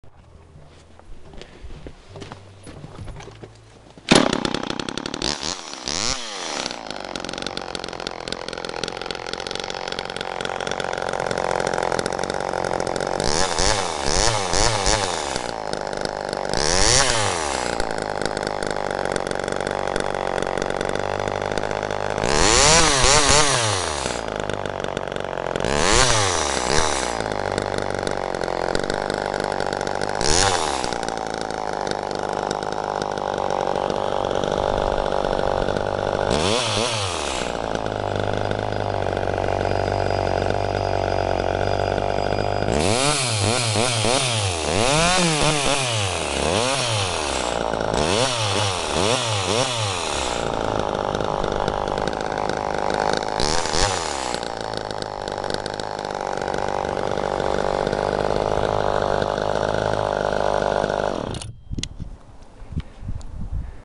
Chainsaw Homelite XL-1Automatic/ Super Mini Test Run
Vintage Homelite XL-1Automatic in great shape, with electronic ignition, start up and runs perfect. Automatic chain oiler works fine. Nice little saw for for your firewood at home or camping etc.